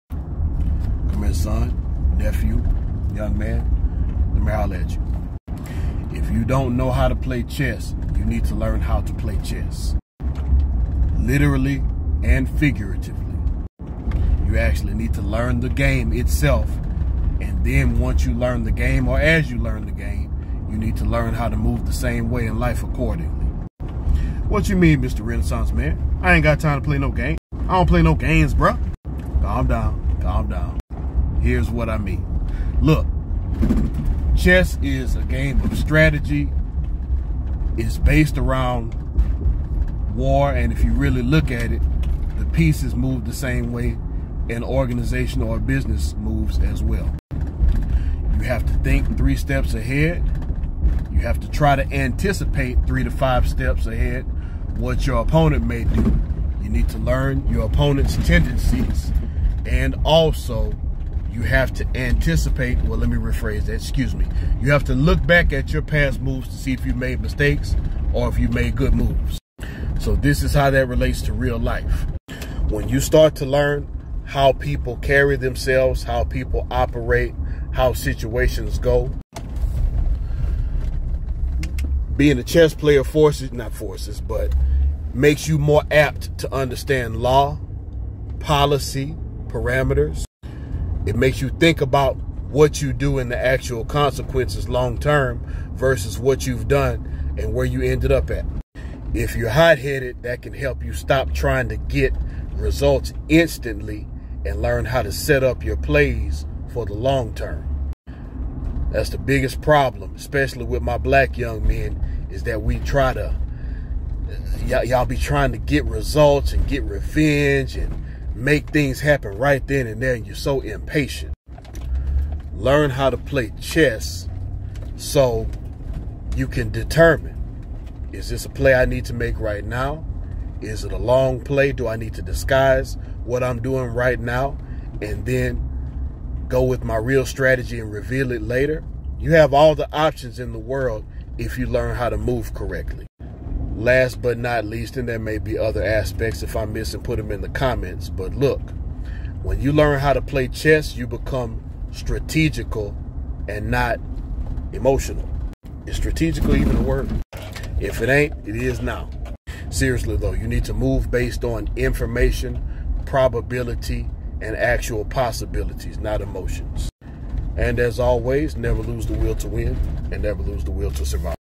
Life is Chess not checkers
Chess is a great tool for learning how to proceed strategically throughout life.